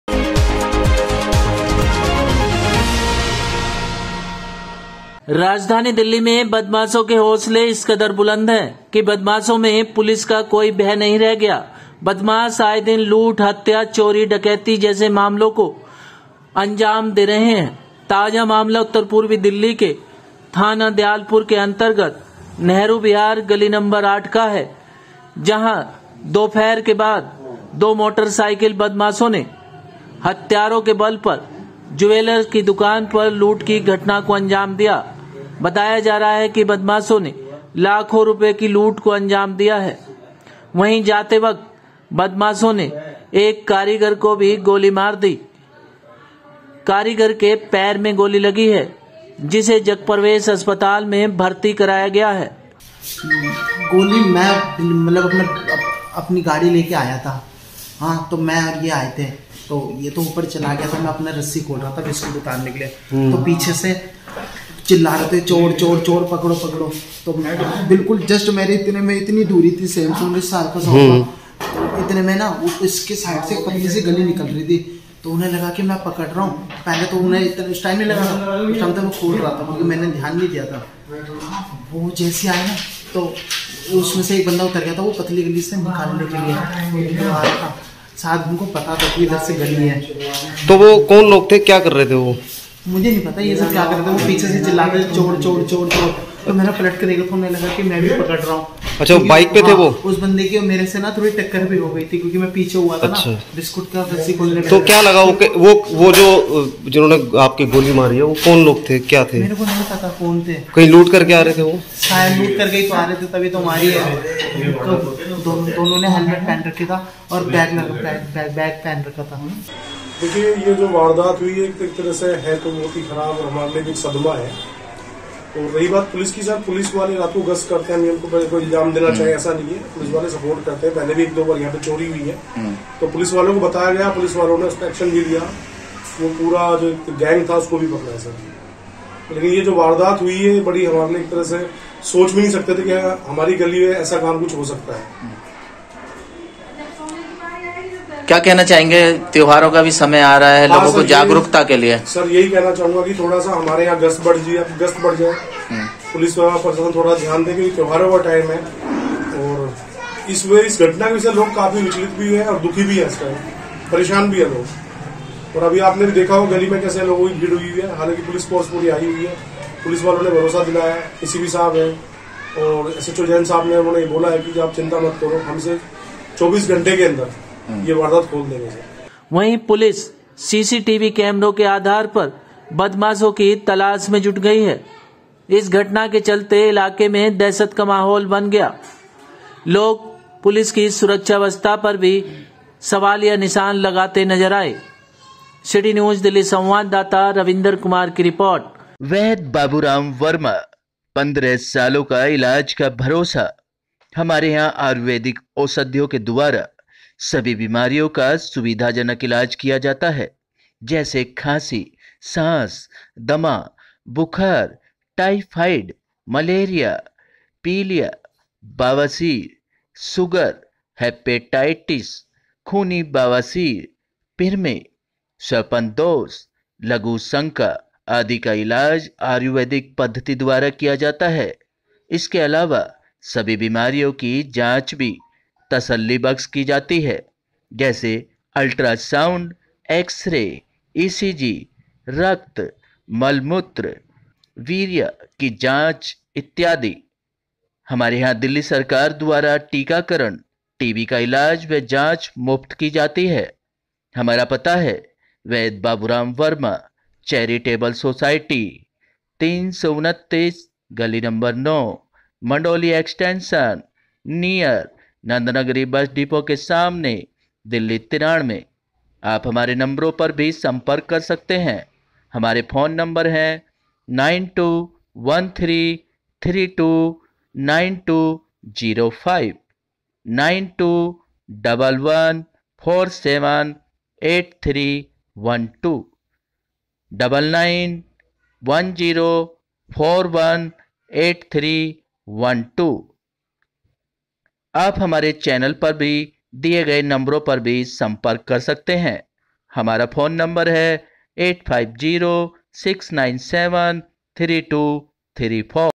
CITY NEWS DELHI -वर्मा ज्वेलर्स की दुकान पर बदमाशों ने हथियारों के बल पर लूट की घटना को दिया अंजाम ।
थाना दयालपुर के अंतर्गत नेहरू विहार वर्मा ज्वेलर्स की दुकान पर बदमाशों ने हथियारों के बल पर लूट की घटना को दिया अंजाम । दुकान पर काम करने वाले एक युवक को बदमाशों ने मारी गोली  ।लाखों की लूट को बदमाशों ने दिया अंजाम । बदमाशों ने 4 राउंड गोलियां भी चलाई । इलाके में दहशत का माहौल बना । खबर लिखे जाने तक कितने की लूट हुई यह साफ नहीं हो पाया । घायल व्यक्ति को जग प्रवेश अस्पताल में किया भर्ती । पुलिस सीसीटीवी कैमरों के आधार पर बदमाशों की तलाश में जुटी । बताया जा रहा है कि बदमाश मोटरसाइकिल पर सवार होकर आए थे । बदमाशों ने हेलमेट पहने हुए थे